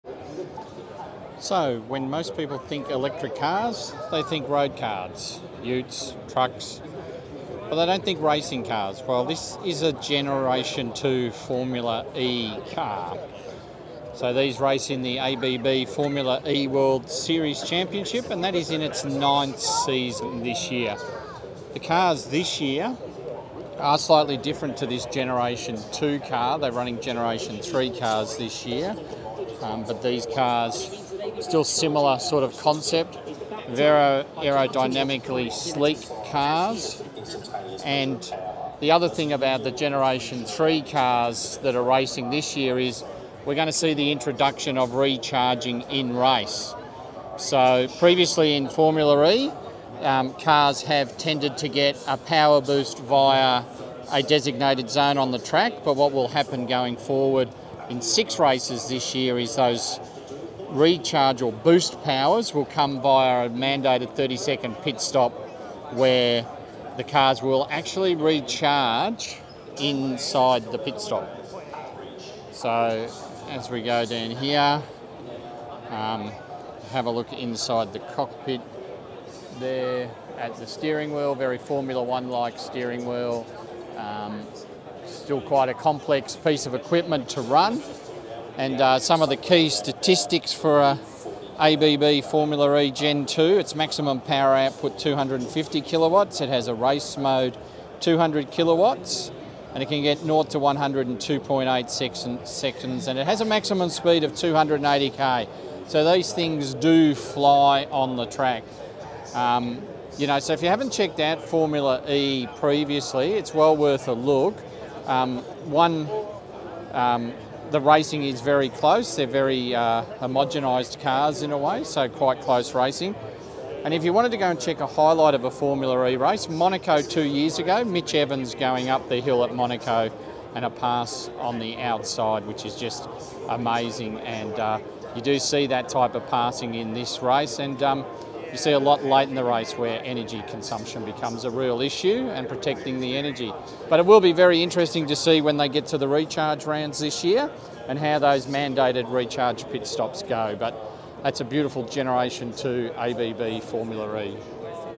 We get up close to a Gen 2 Formula e car
We take a walk around a Gen 2 ABB Formula e car and talk about some. Of the existing rule changes for the 9th season, including recharge pit stops.